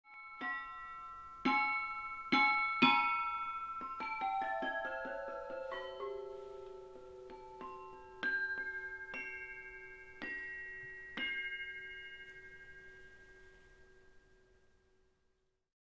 What's That Sound #1 REVEAL
Here's the answer to What's That Sound #1.
Pipe gamelan! Galvanized steel tubes, tuned chromatically. F4-F7 range.